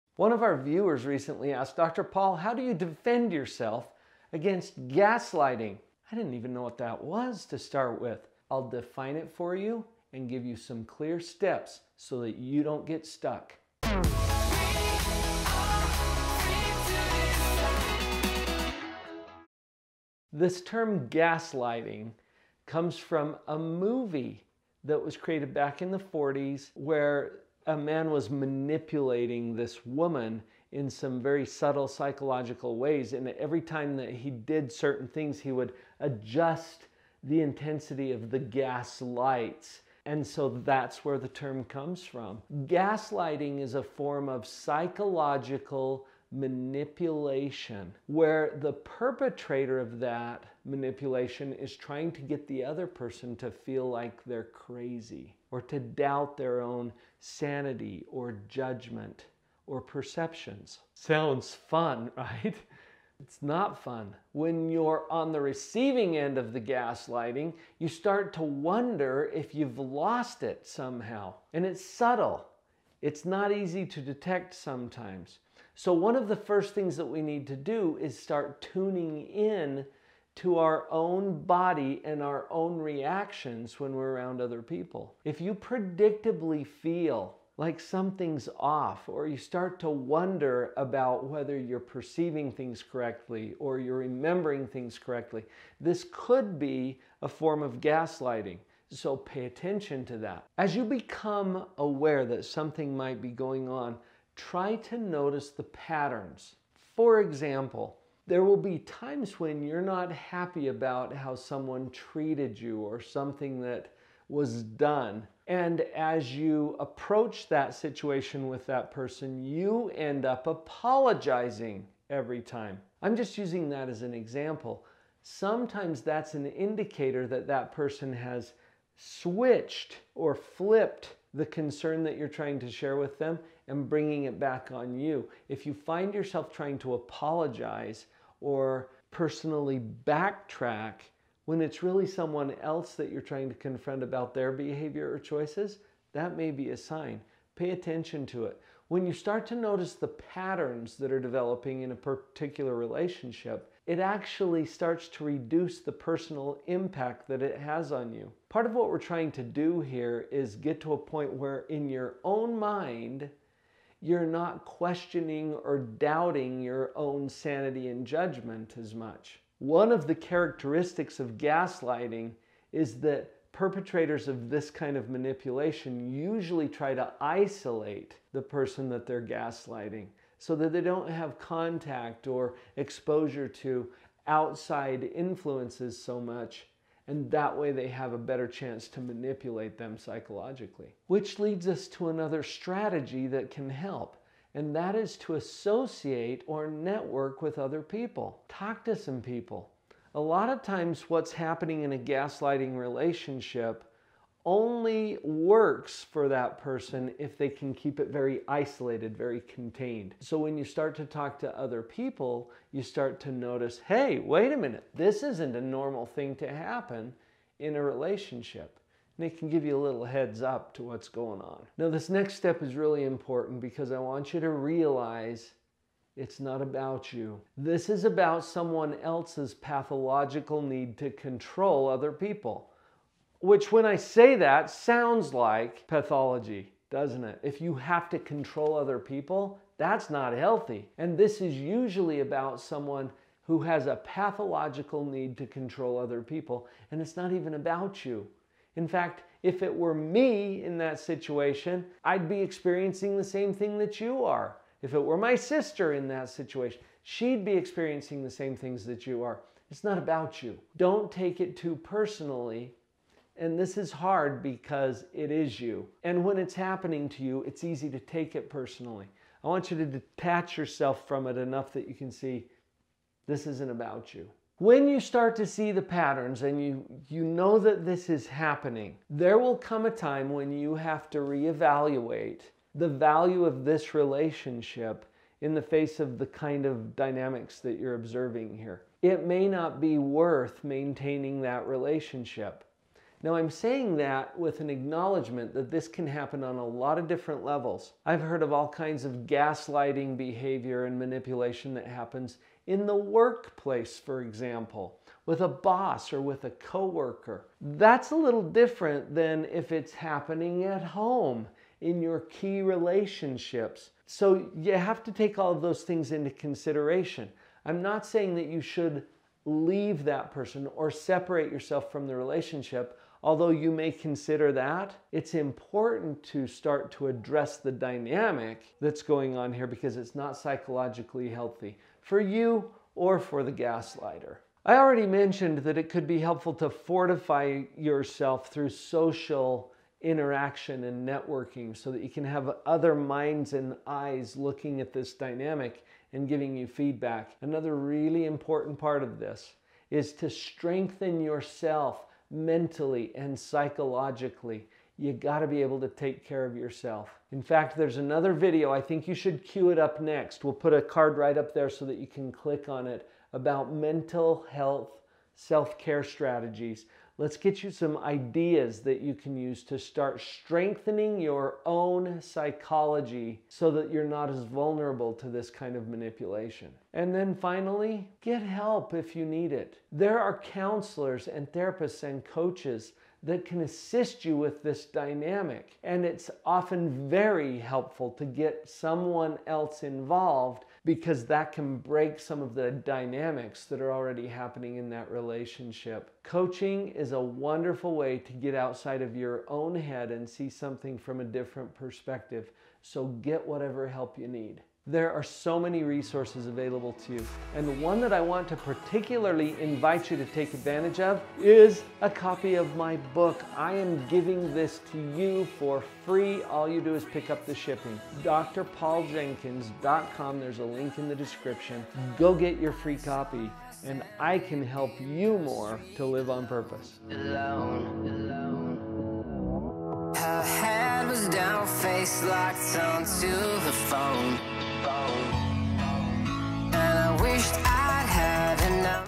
How To Defend Yourself Against Gaslighting
Gaslighting is a term you don't hear very often, but it's actually a common form of manipulation and abuse that shouldn't be ignored. It might be happening to you without your knowledge! In today's episode, I'll explain what gaslighting means, how you can detect it, and how you can break free from this kind of manipulative relationship.

00:20 What is gaslighting?
01:15 Pay attention to these signs
02:28 Key trait of perpetrators
03:14 Associate with other people
03:42 It's not about you
04:40 Is it worth it?
05:35 Strengthen yourself
06:17 Seek help

Dr. Paul Jenkins